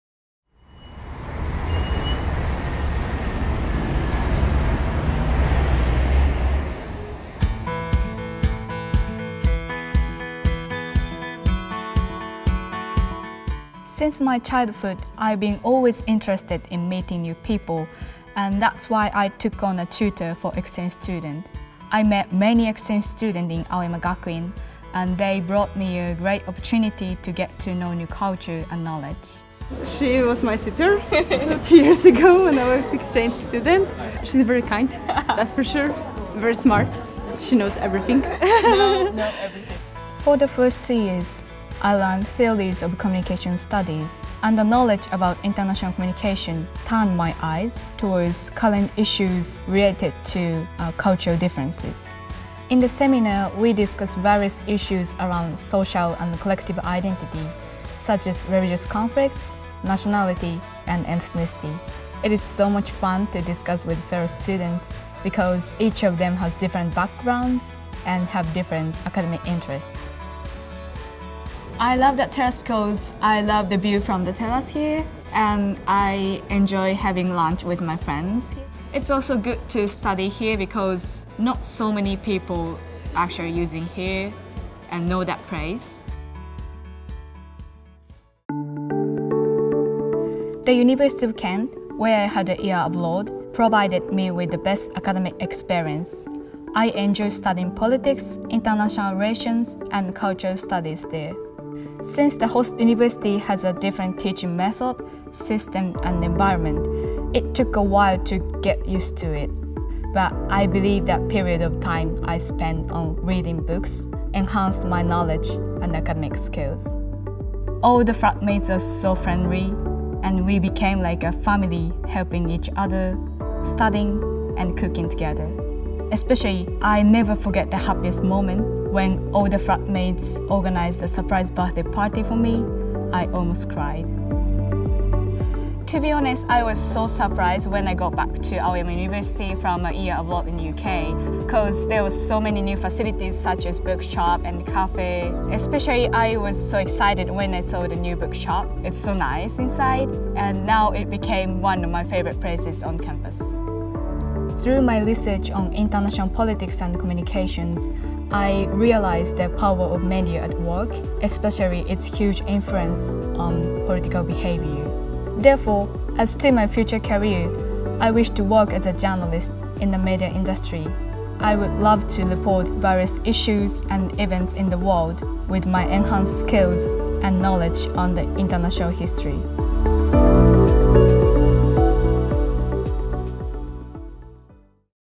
【AGU LiFE】Spotlighting AOGAKUJIN #001
With the aim of broadening the pursuit of knowledge and fostering future workers capable of living up to the challenges of the times, AGU offers a diverse range of 11 undergraduate faculty/colleges/schools composed of 25 departments as well as 12 deeply specialized graduate schools. Interviews were held with our students and alumni to introduce how they have made use of our education to achieve growth and demonstrate their full potential.